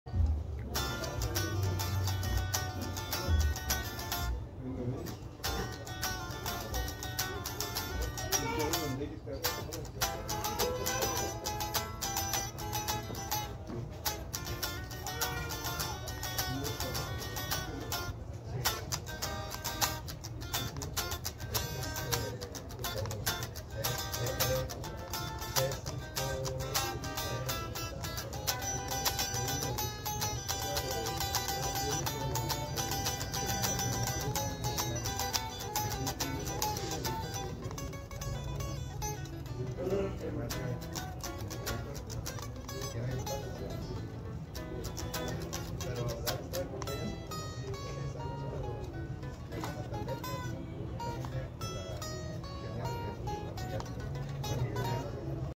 Pascual: Funky jam con una Fender American Stratocaster de 2600 dólares (2024)
El 5 de octubre de 2024 fui a Guitar Center El Paso y calé algunos instrumentos. Cheers.